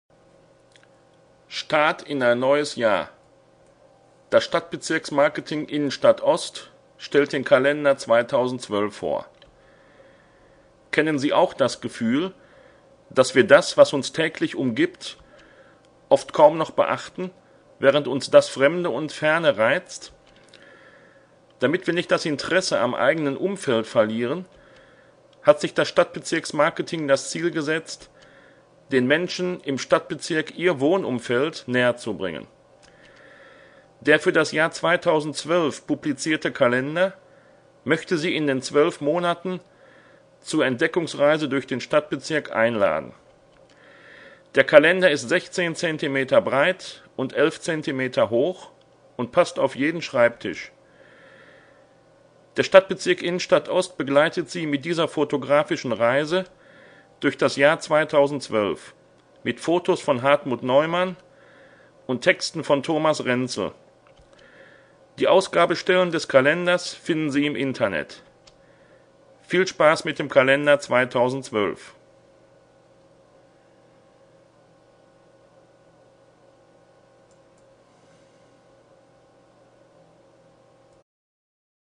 Das Stadtbezirksmarketing Innenstadt Ost der Stadt Dortmund stellt den Kalender 2012 vor
Der für das Jahr 2012 publizierte Kalender möchte Sie in den 12 Monaten zur Entdeckungsreise durch den Stadtbezirk einladen.